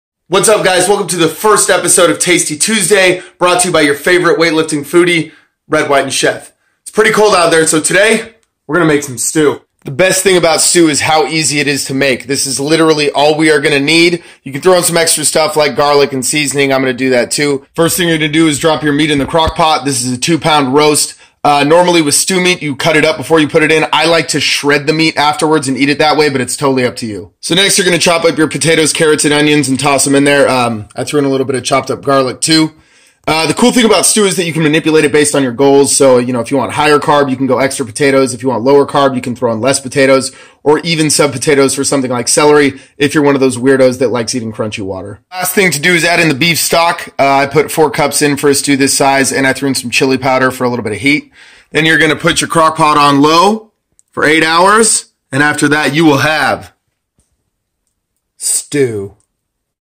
DELTA Performance Nutrition TastyTuesday Episode 1: Beef Stew
Nutrition is the foundation for performance, but it also happens to be one of the more difficult aspects of performance to maintain consistent compliance, so our goal here is to take some of the thinking out of it for y’all by providing some great meal ideas!

Since the weather is starting to get a little chilly out there, first on the menu is BeefStew. Beef stew is a great post training meal filled with all the essential macro nutrients of protein, fat, and carbs. If you use the right ingredients, you’ll get the right micro nutrients too!

Here's the recipe:
-2lb top sirloin or chuck roast (Grass fed if possible for higher nutrient density).
-3 russet potatoes (veggies organic if possible to avoid gut irritants and higher nutrient density).
-4 large carrots
-1 large white onion
-6 cloves garlic (minced)
-4 cups beef stock (1 box)
-Chili powder to taste

Tracking your macros? Fear not! Simply weigh and log everything before cooking, and divide the total macros based on how many servings you make.

Dial in your nutrition, improve your performance.